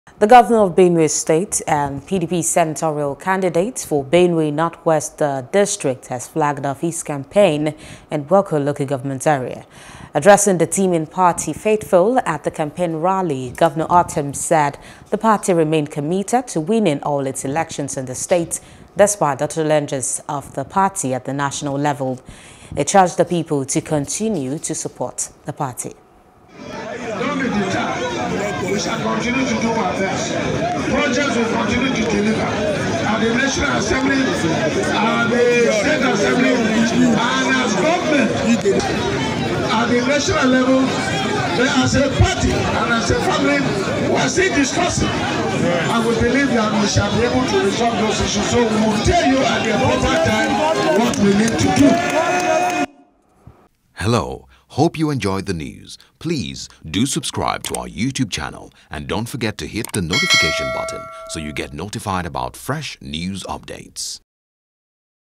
2023 Elections: Ortom Flags-Off Benue North-Westsenatorial Campaigns In Gboko | NEWS
The Governor Of Benue State And PDP Senatorial Candidate For Benue North-West District Has Flagged-Off His Campaign In Gboko Local Government Area.